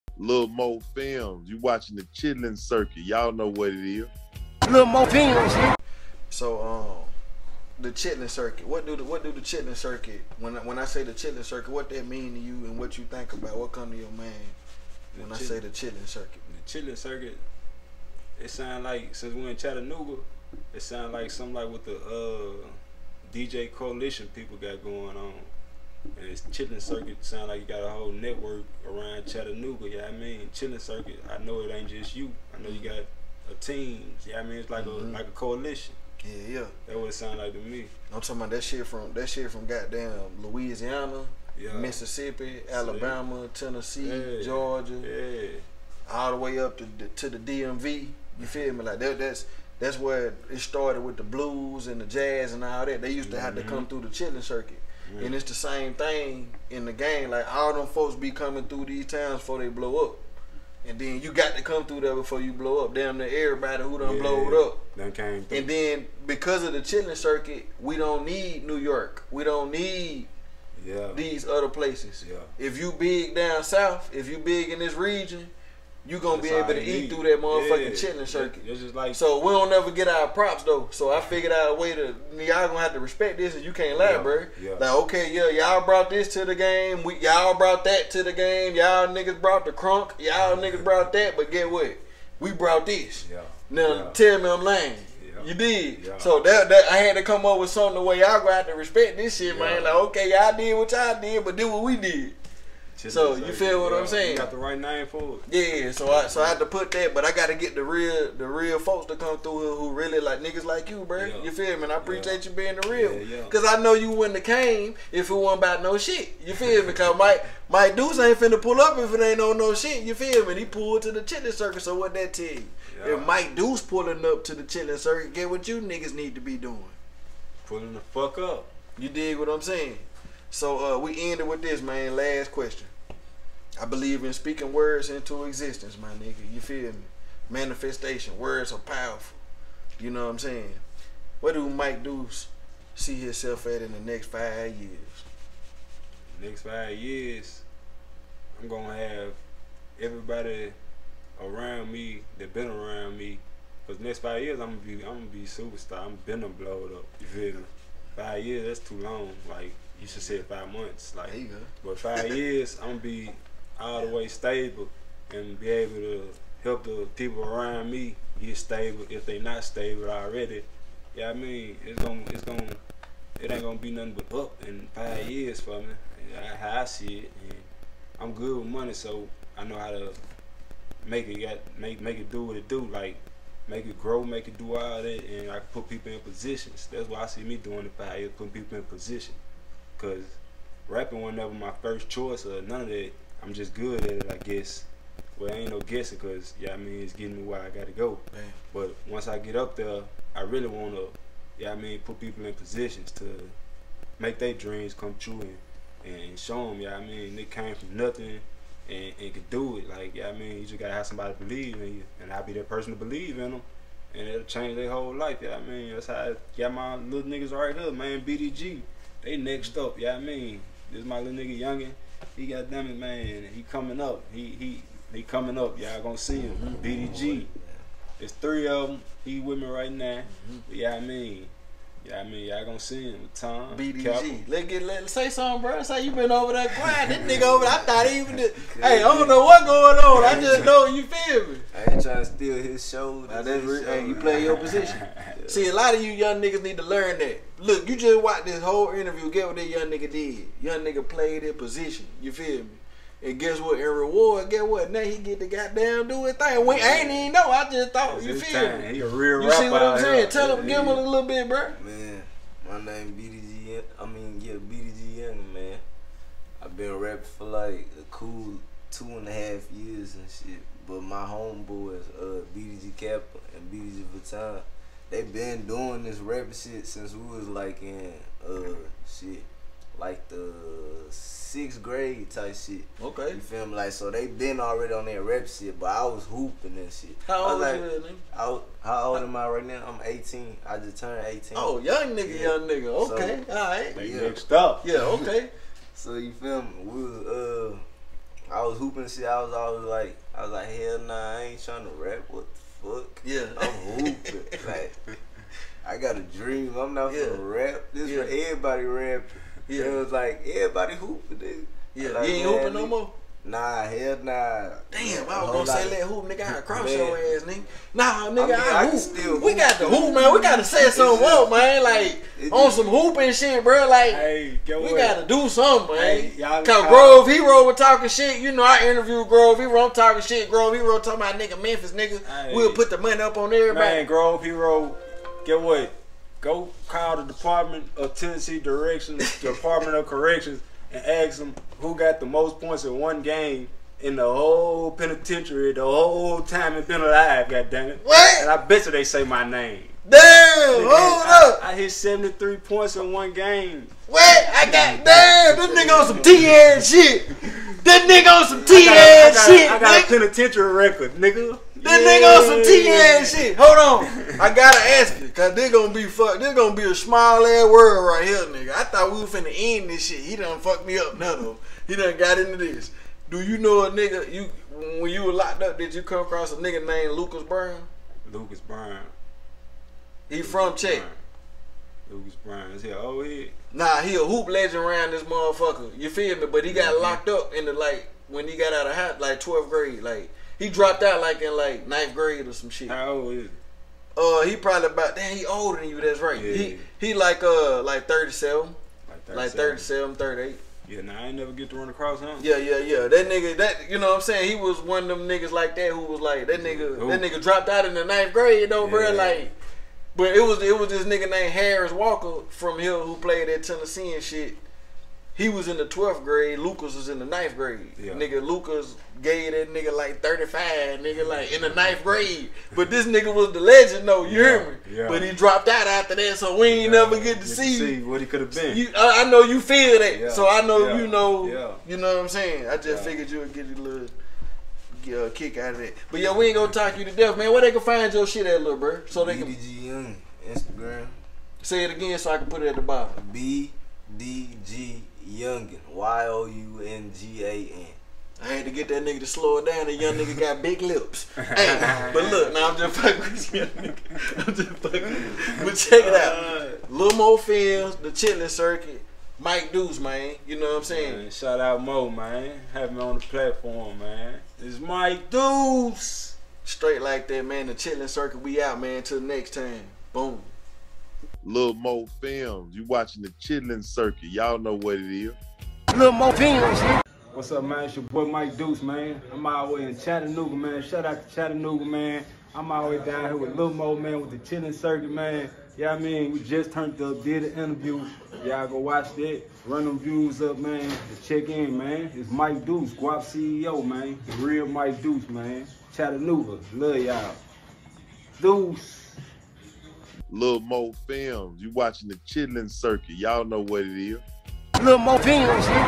Mike Duece Pt.7 T-HEAD Memphis Legend,Grove Hero PRISON Basketball mikeduece T-head
Mike Duece Introduces BDG Youngin, Sends A Message To Grove Hero and T-Head In Memphis.